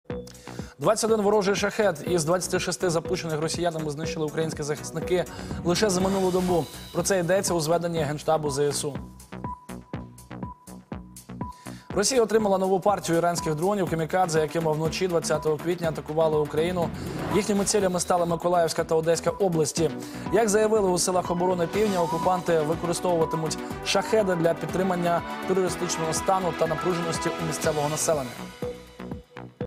ЗСУ минулої доби знищили 21 «шахед»
Протягом минулої доби противник завдав 3 ракетних та 57 авіаційних ударів, зокрема, застосував 26 БПЛА типу „Шахед-136“. 21 з них — знищено нашими захисниками.